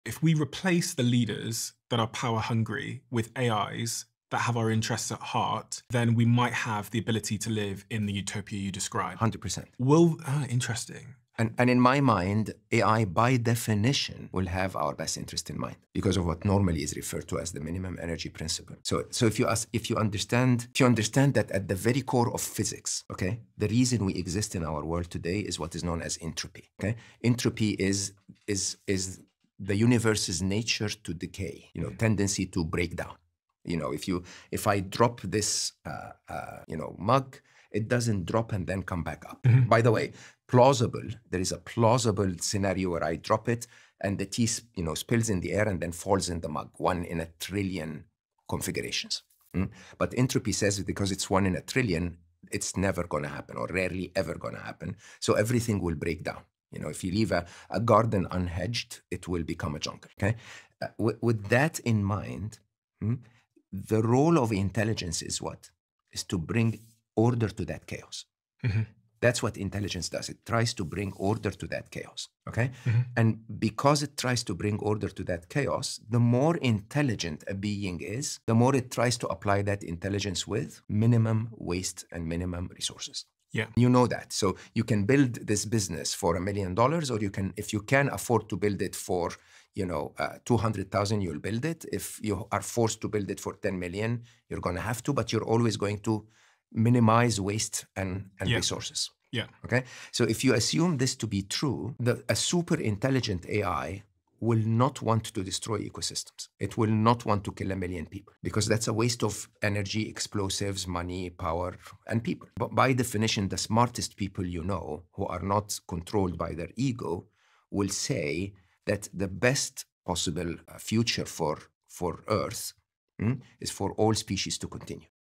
AI Utopia: Can Artificial Intelligence Save Earth?
Could AI leaders, driven by efficiency, create a utopia? A super-intelligent AI would minimize waste, preserving ecosystems and human life, unlike ego-driven leaders. The best future is for all species to continue.